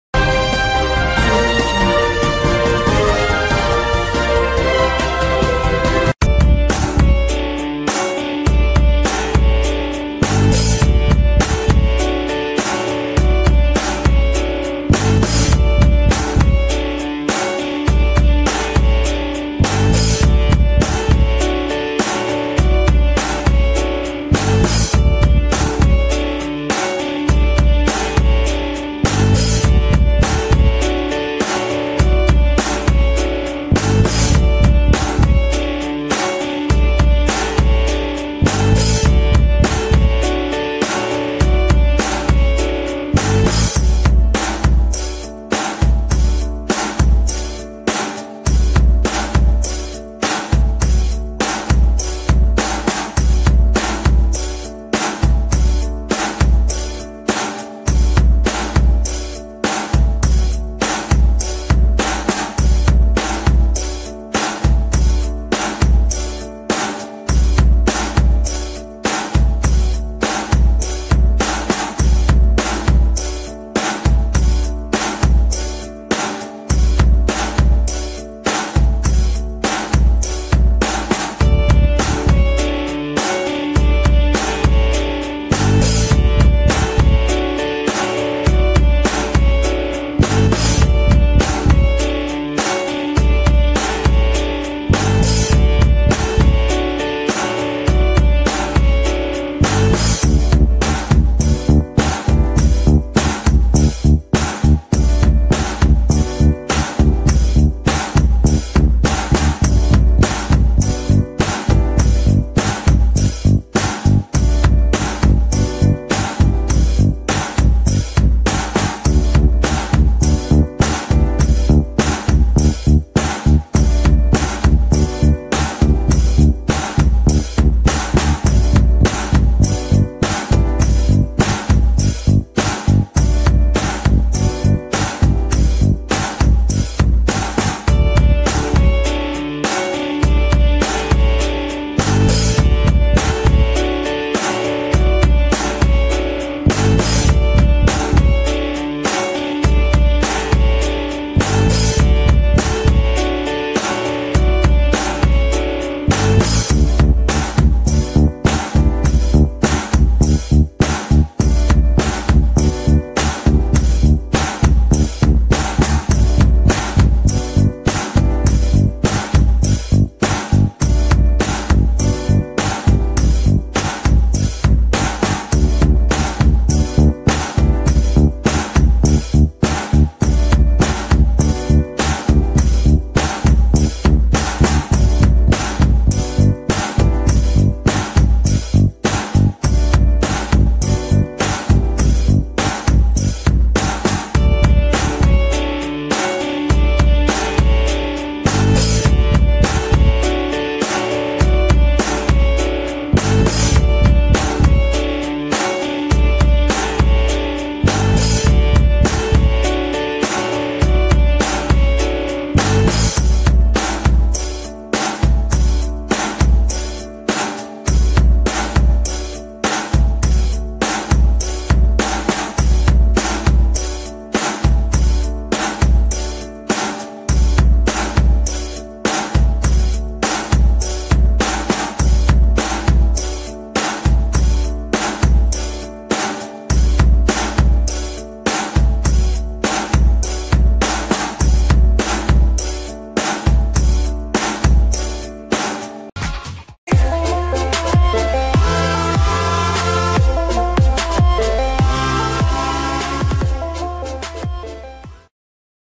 Eve Muirhead bombarded with ‘beautiful’ and ‘hot’ tweets after Winter Olympics curling win
► Twitter users bombarded the site with messages this afternoon as the Scot, 27, delivered the winning...
► PLL:
► Photo & Content Source :